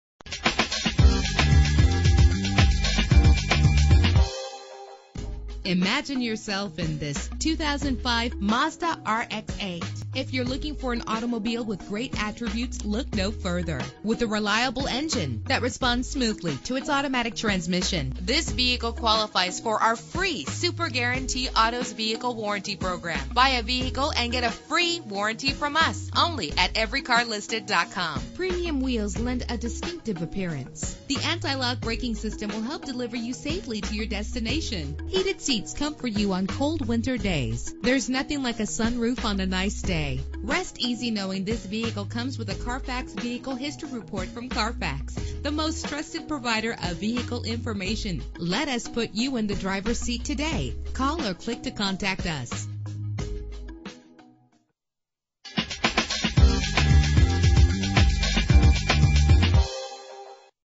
2005 Mazda RX-8 - Antioch IL
Year: 2005
Make: Mazda
Model: RX-8
Trim: Shinka
Engine: 1.3 liter
Transmission: 4-Speed Automatic
Color: Red
Mileage: 25452
Address:
118 Rt 173